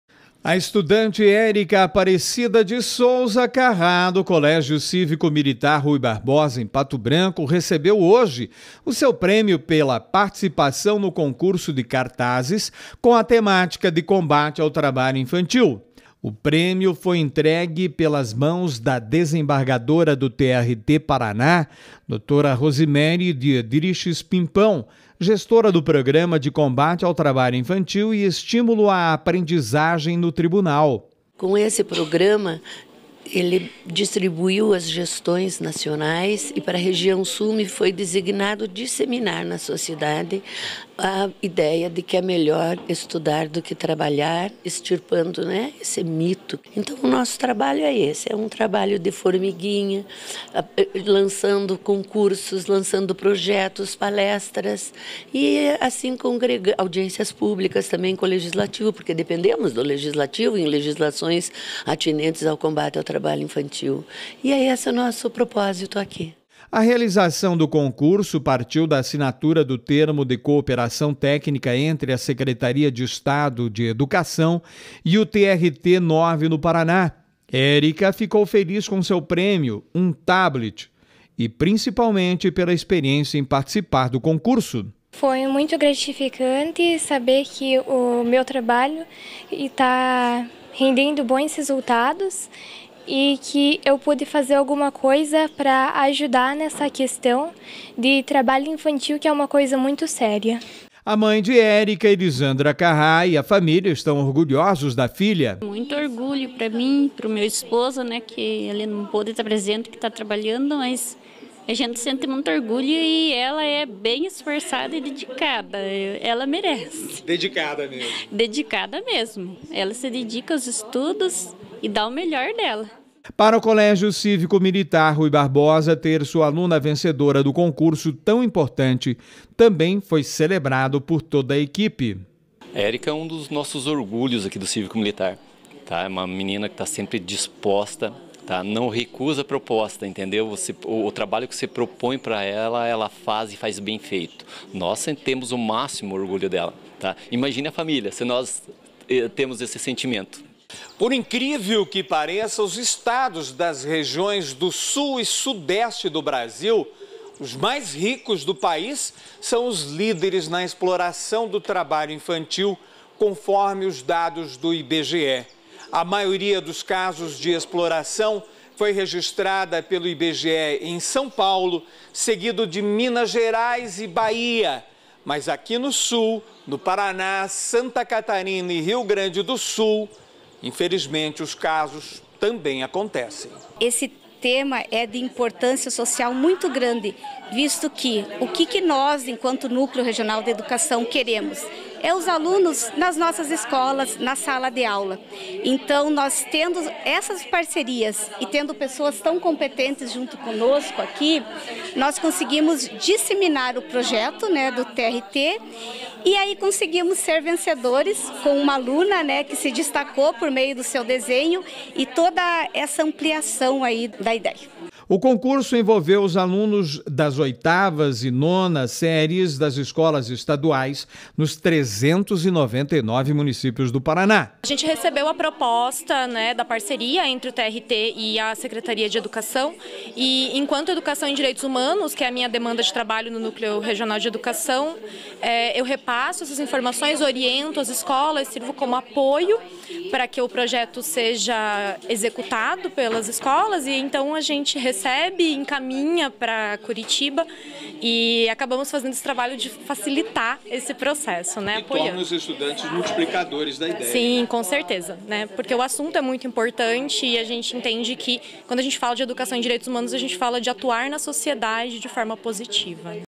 Estudante pato-branquense vence concurso do TRT-PR sobre trabalho infantil
Educação: Estudante do Colégio Cívico Militar de Pato Branco recebeu hoje (05) o prêmio pela participação no concurso de cartazes com a temática sobre o trabalho infantil.

Texto de: Betto Rossatti.